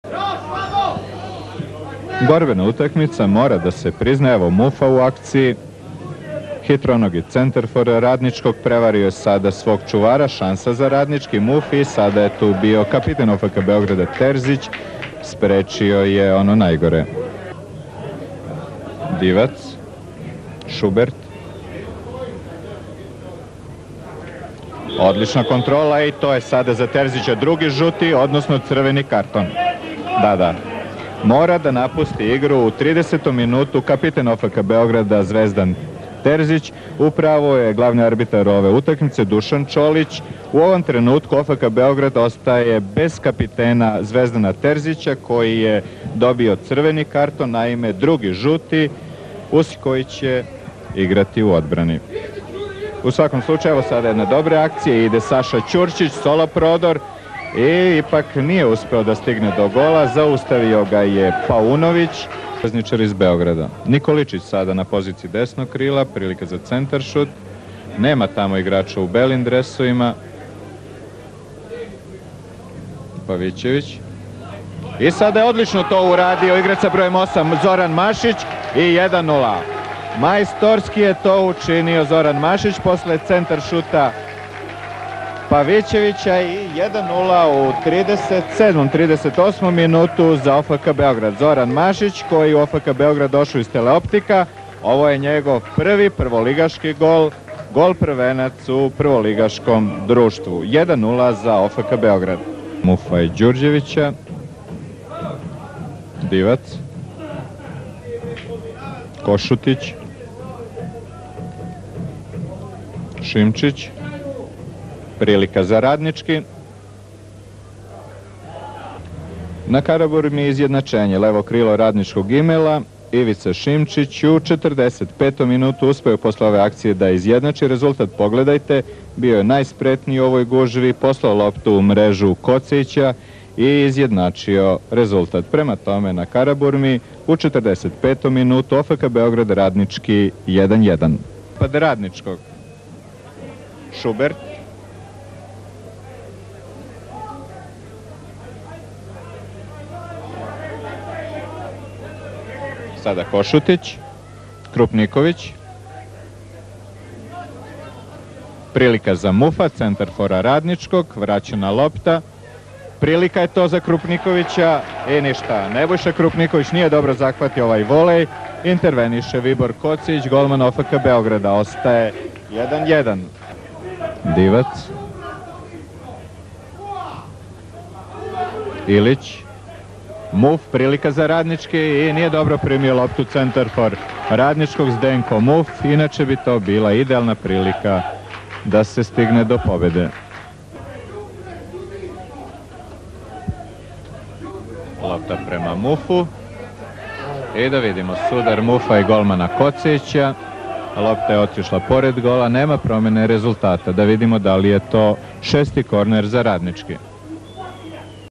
OFK Beograd - Radnicki Novi Beograd 1:1, Sezona 1992/93.mpg
3.Kolo Prvenstva Jugoslavije u fudbalu , Sezona 1992/93
Omladinski Stadion na Karaburmi, 2000 gledalaca
Strelci:
1:0 Mašić 38.min
1:1 Šimičić 45.min